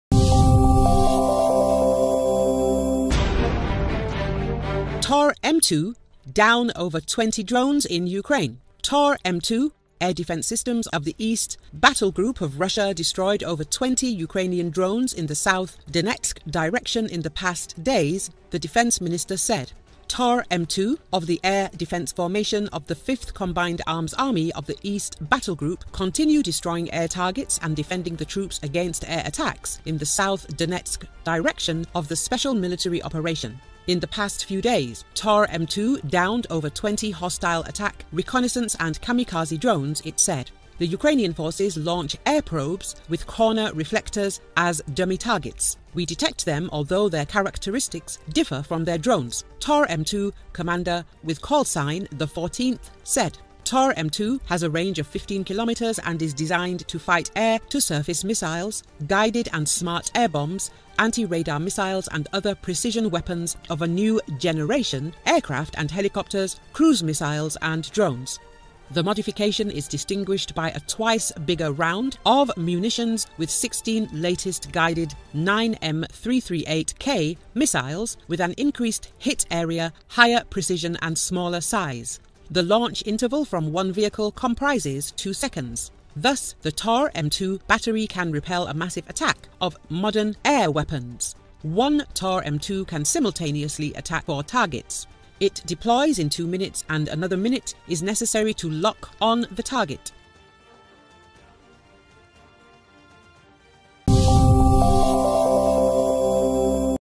Tor-M2 systems down over 20 drones in Ukraine's Donetsk region
Tor-M2 air defense systems of the East battlegroup of Russia destroyed over 20 Ukrainian drones in the south Donetsk direction in the past days, the Defense Ministry said.
"Tor-M2 of the air defense formation of the 5th combined arms army of the East battlegroup continue destroying air targets and defending the troops against air attacks in the south Donetsk direction of the special military operation. In the past days, Tor-M2 downed over 20 hostile attack, reconnaissance and kamikaze drones," it said.
The Ukrainian forces launch air probes with corner reflectors as dummy targets. "We detect them although their characteristics differ from drones," Tor-M2 commander with call sign the Fourteenth said.
Tor-M2 has a range of 15 km and is designed to fight air-to-surface missiles, guided and smart air bombs, antiradar missiles and other precision weapons of a new generation, aircraft and helicopters, cruise missiles and drones. The modification is distinguished by a twice bigger round of munitions with 16 latest guided 9M338K missiles with an increased hit area, higher precision and smaller size. The launch interval from one vehicle comprises two seconds. Thus, a Tor-M2 battery can repel a massive attack of modern air weapons. One Tor-M2 can simultaneously attack four targets. It deploys in two minutes and another minute is necessary to lock on the target.